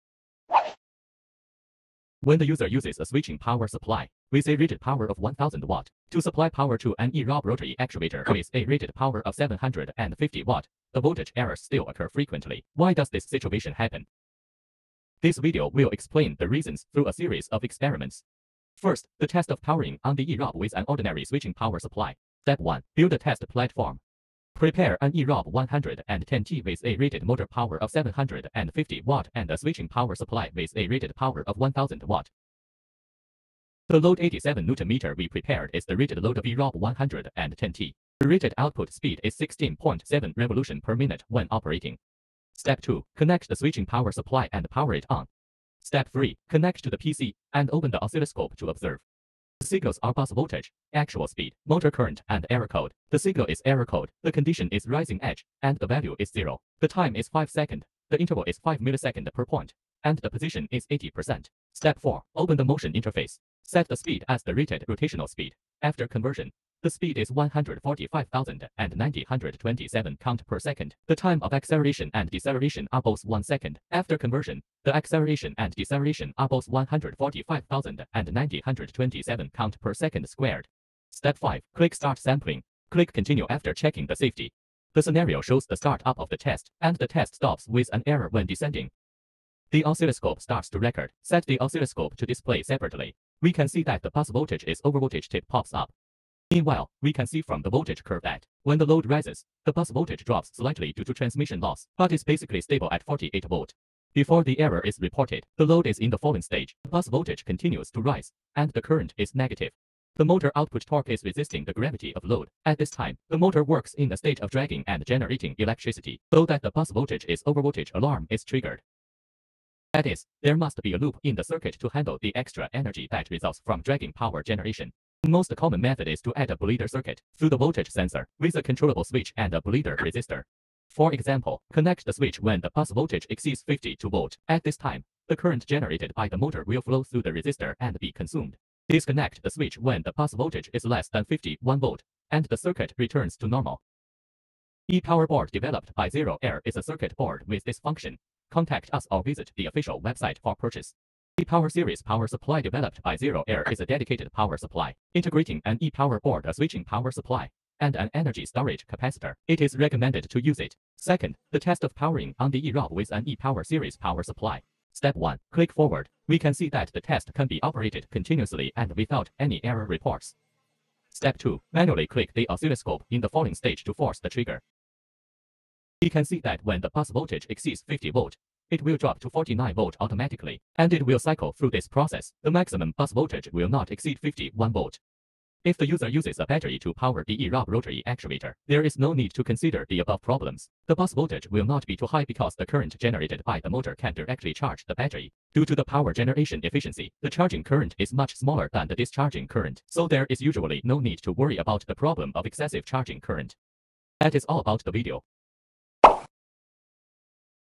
The reason why ordinary switching power supply cannot be used as the power supply of eRob?
ZeroErr ePower Board & ePower
Dedicated power supply of eRob rotary actuator.